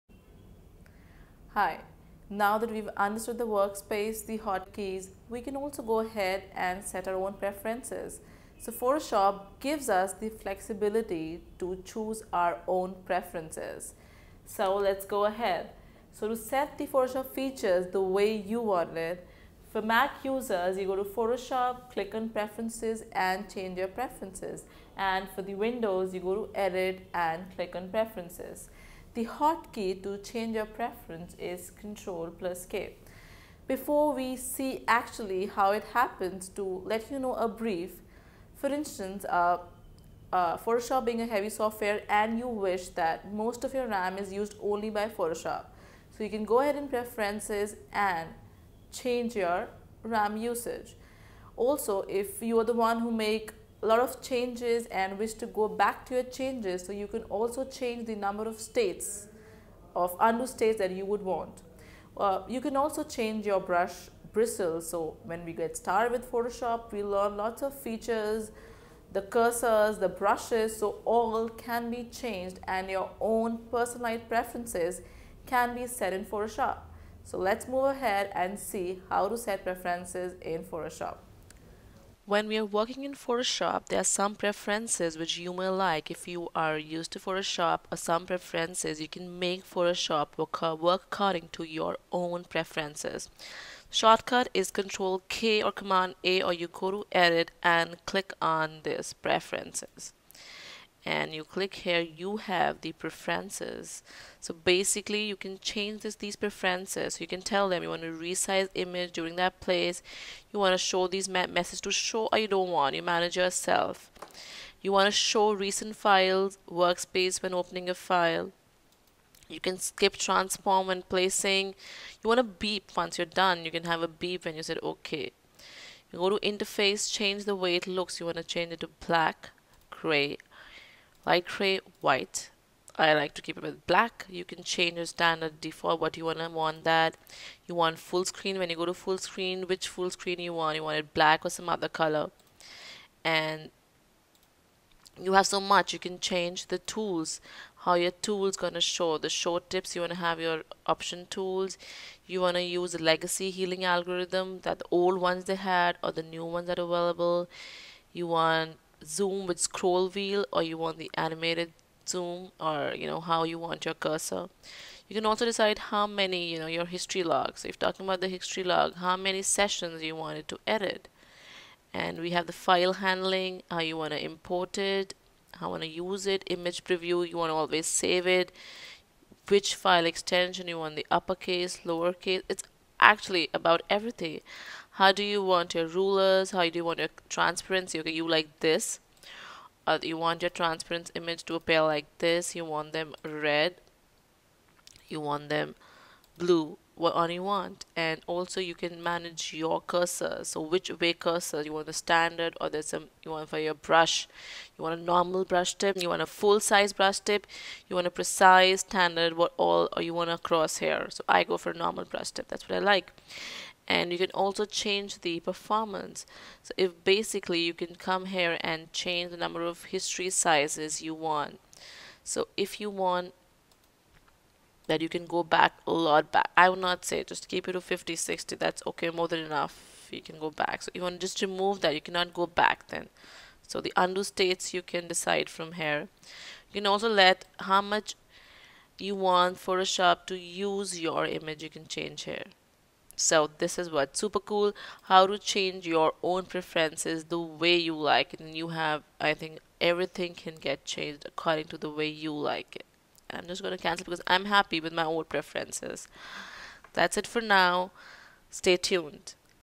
Photoshop - Setting the Preferences
Lecture By: Ms. Ayushi Nangru, Tutorials Point India Private Limited